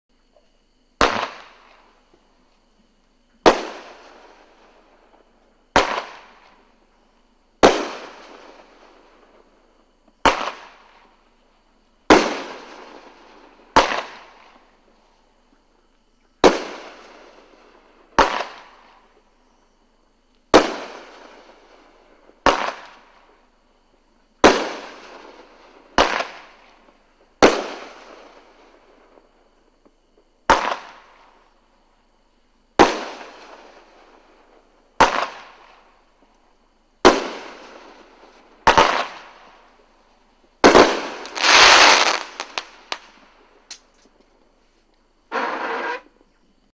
Ultimate Warrior
500 gram cake with tails to large breaks.